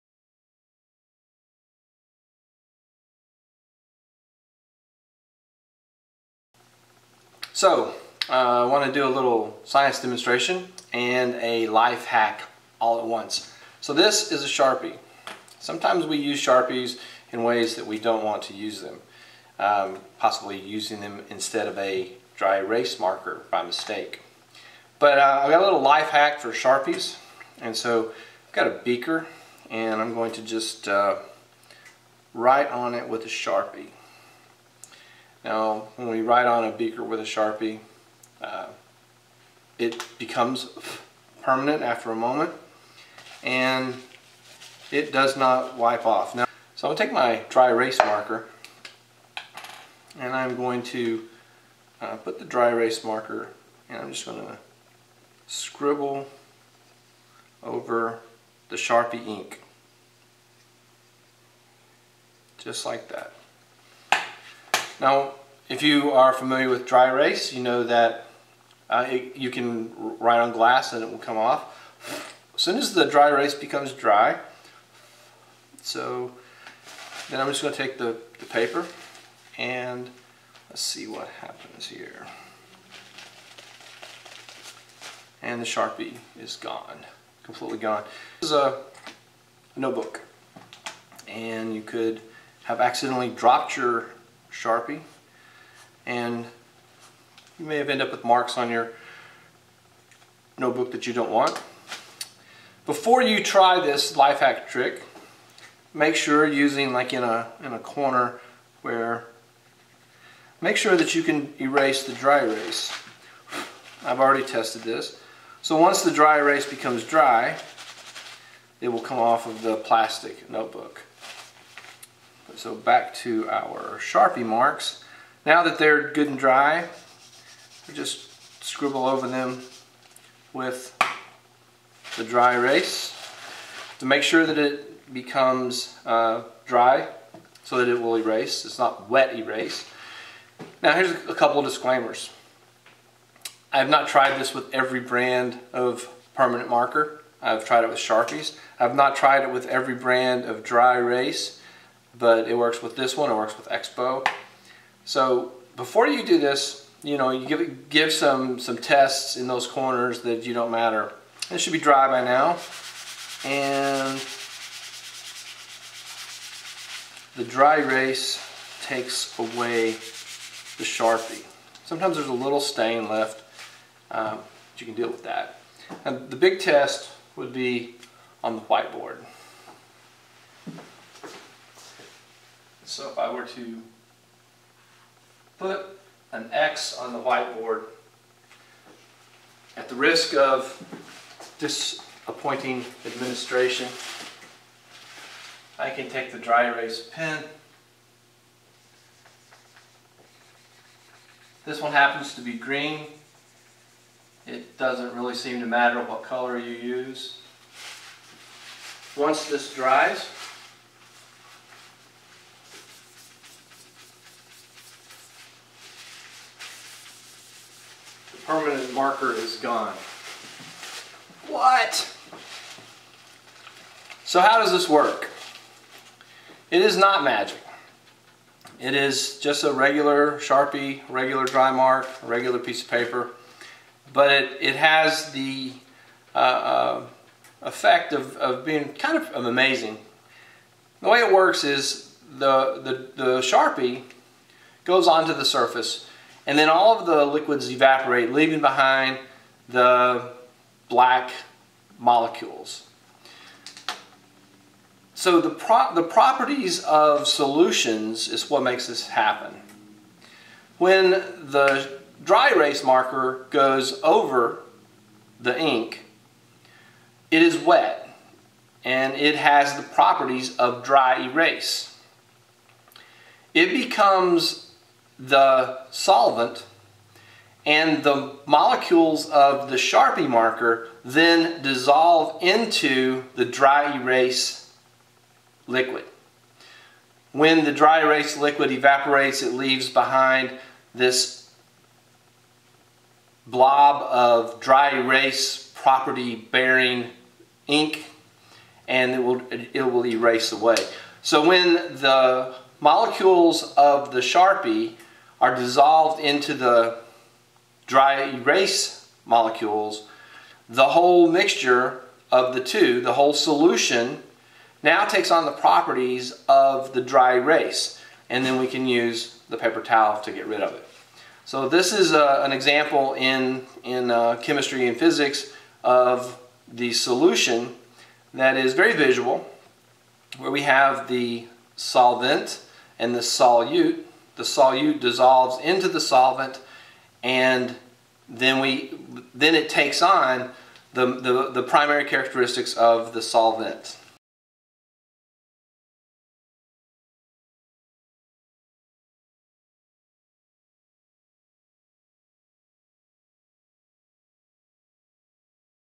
Sharpie Life Hack Science (Types of Matter: Solutions)
Life-hack! Erase Sharpie® and learn why it works!

Solutions, solvents, solutes. What could be more fun?

Notice:

Video shot at Lowell High School, Tricreek School Corporation, Lowell, Indiana by/with students in the Introduction to Chemistry and Physics class.

Lowell High School,  Tri-creek School Corporation, Lowell, Indiana and The State of Indiana are not responsible for the production or content of this video.

The students appearing in this video did so with express consent from their legal guardians.